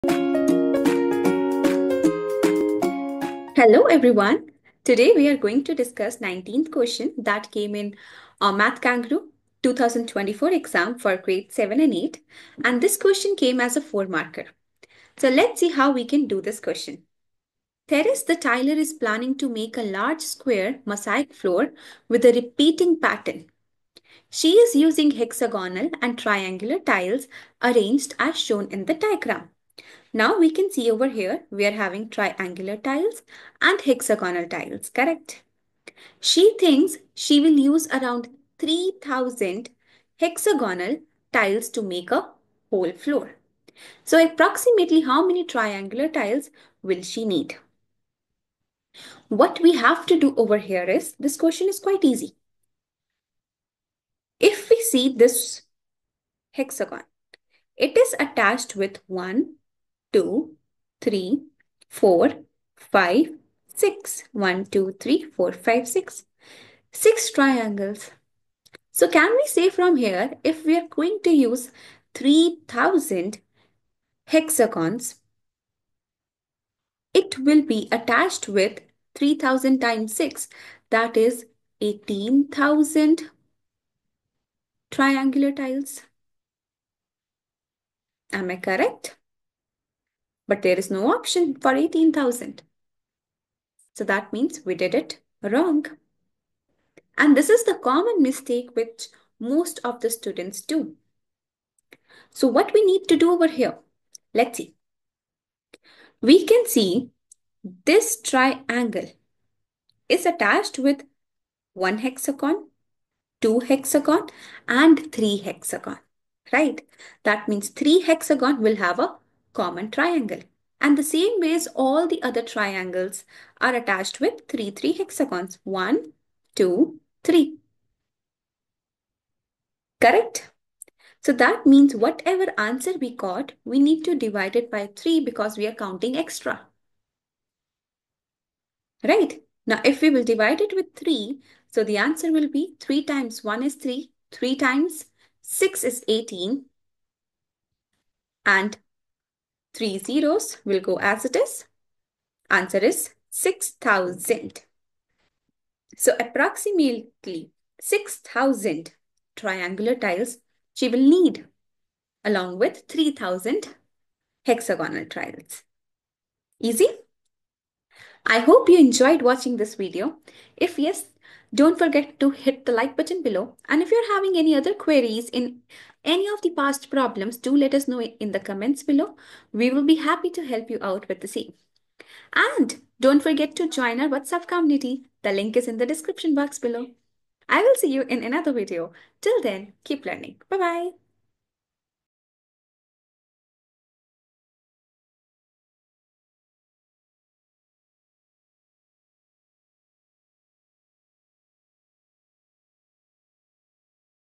Math Kangaroo 2024 Solutions I Level 7-8 I Problem 19
Math Kangaroo 2024 Solutions | Level 7-8 | Problem 19

Question 19:
Therese the tiler is planning to make a large square mosaic floor with a repeating pattern. She is using hexagonal and triangular tiles arranged as shown in the diagram. She thinks she will use 3000 hexagonal tiles to make the whole floor. Approximately how many triangular tiles will she need?